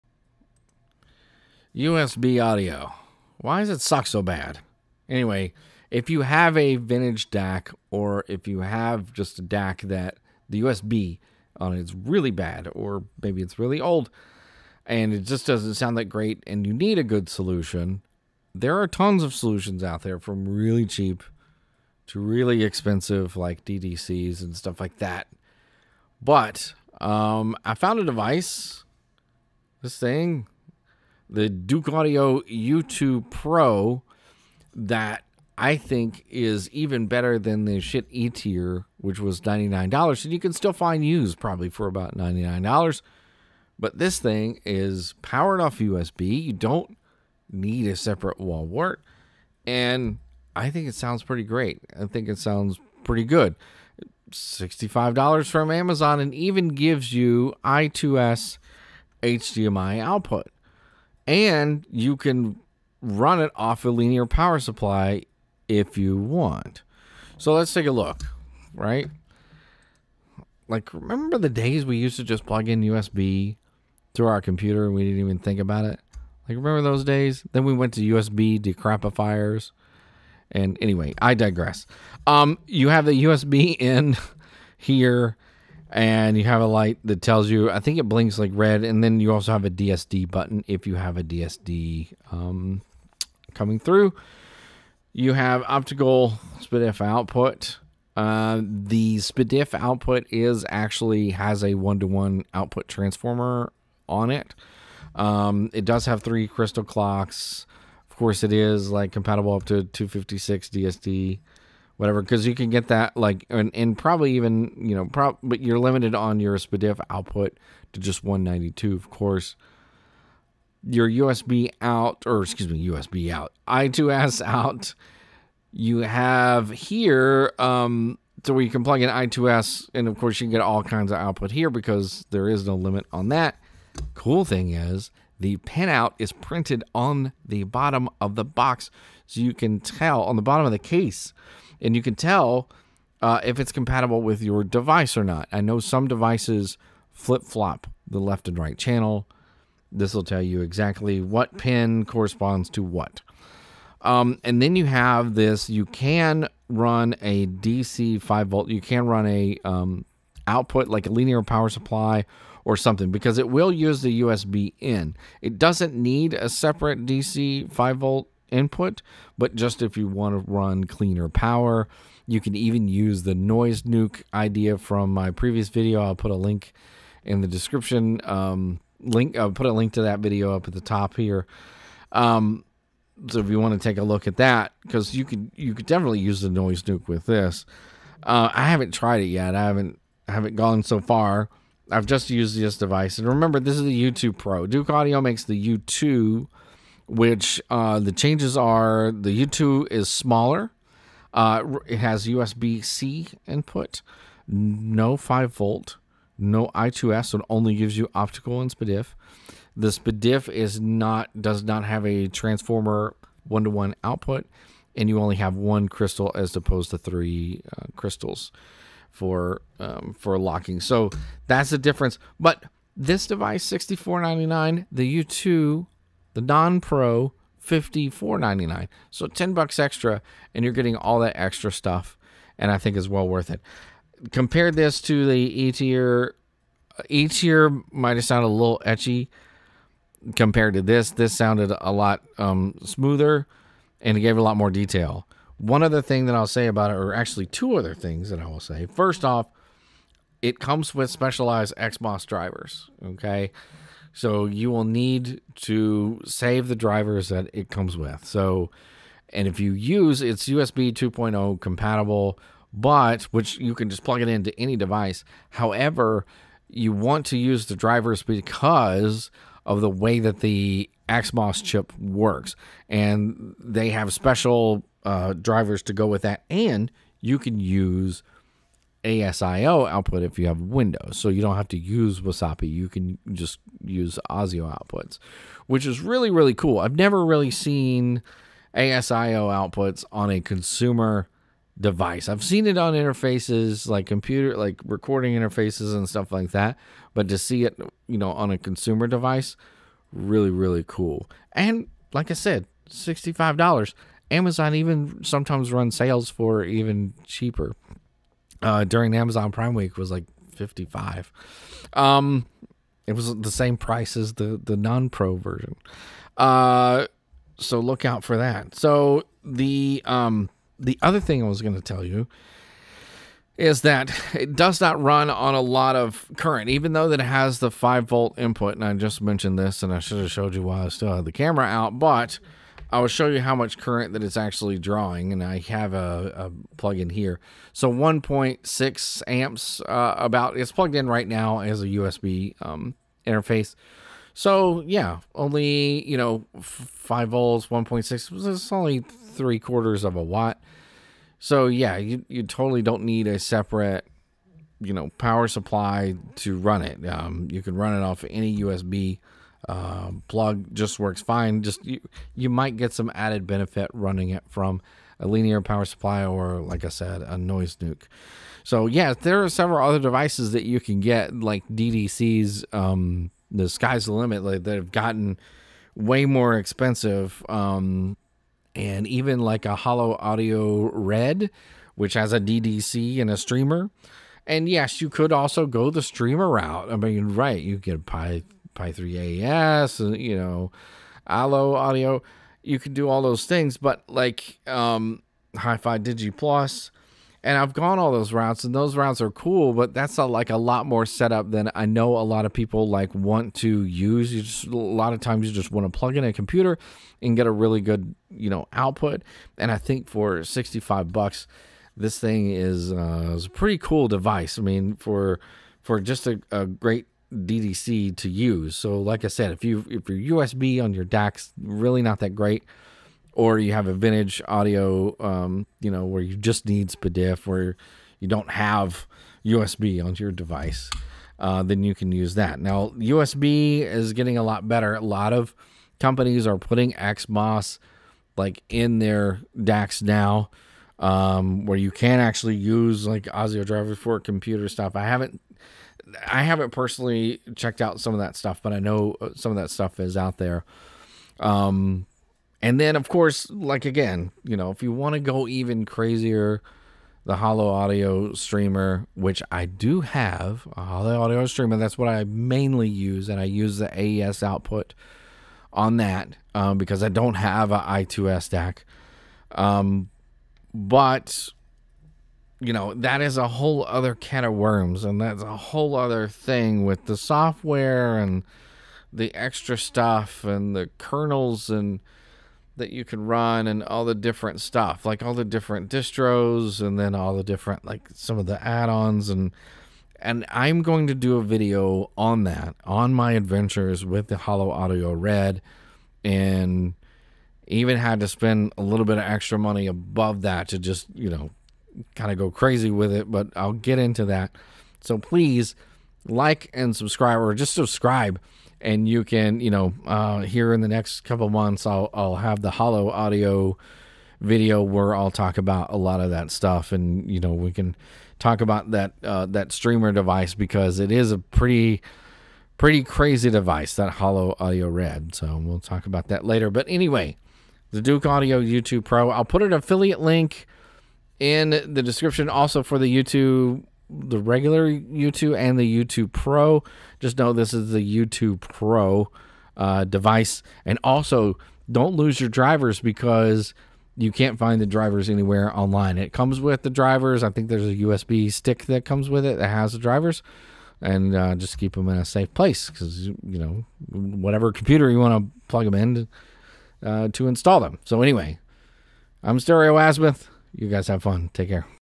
USB to SPDIF and I2S - Douk U2 Pro
USB DDC to use USB to SPDIF, Toslink, and I2S and also use ASIO.

Douk U2 Pro

Douk U2